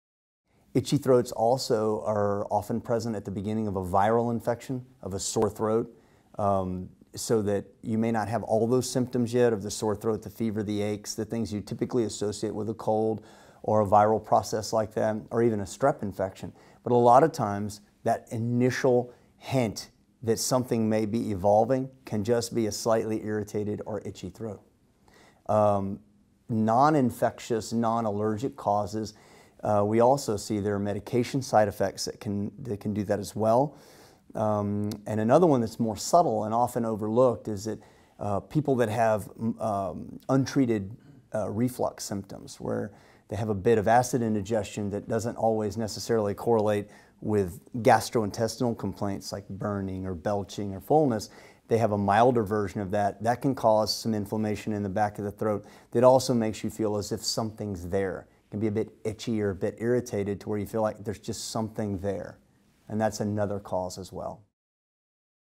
Can an Itchy Throat Be Caused by a Virus or Acid Reflux?
Yes, an itchy throat can be caused by acid reflux or a viral infection, but that's not all. In this video, Patrick Brown, MD, from Methodist Ambulatory Surgery Hospital explains these and other triggers of throat irritation.